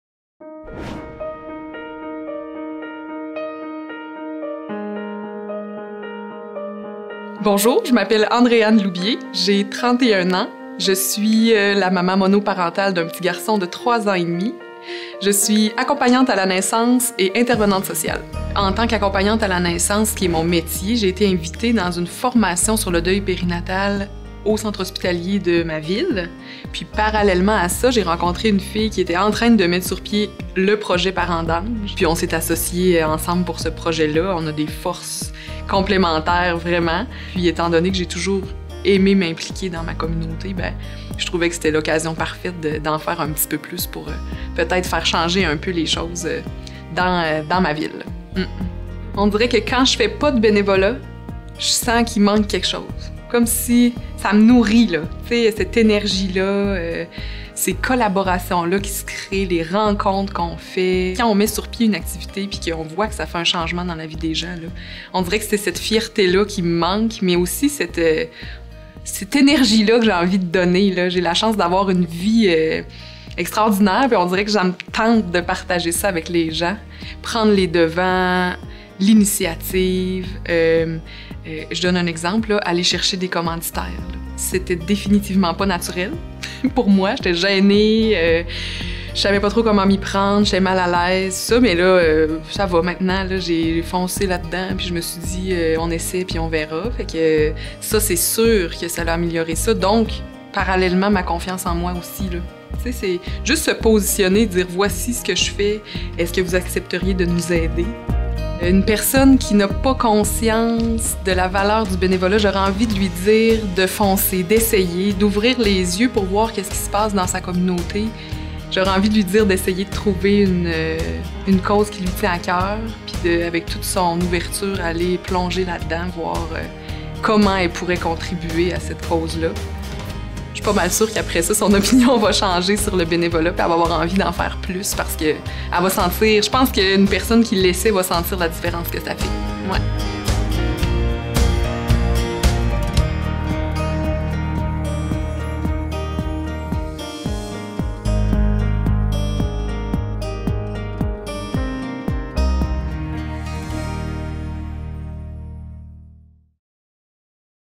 Elles s'impliquent | Andréane Loubier
Nous vous présentons Andréane Loubier, une accompagnante à la naissance également maman monoparentale. Elle a choisi de s'associer bénévolement à l'organisme Parents d'Anges Beauce-Etchemin, qui se spécialise en deuil périnatal.